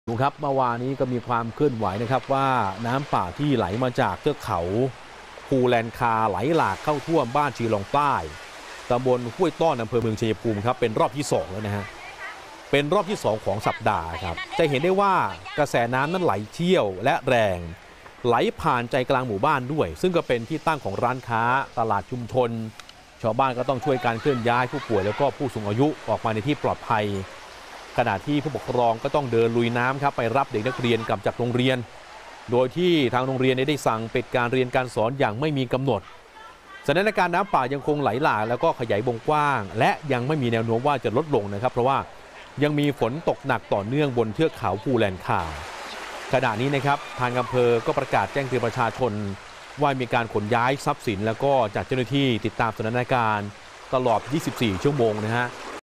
เกิดเหตุน้ำป่าไหลหลากเข้าท่วมหมู่บ้านในอำเภอบ้านเขว้า จังหวัดชัยภูมิ
เกิดเหตุน้ำป่าไหลหลากเข้าท่วมหมู่บ้านในอำเภอบ้านเขว้า จังหวัดชัยภูมิ ระดับน้ำไหลเชี่ยวและแรง ระดับน้ำท่วมสูงกว่า 1 เมตร ชาวบ้านต้องเร่งเคลื่อนย้ายผู้ป่วย และ คนชรา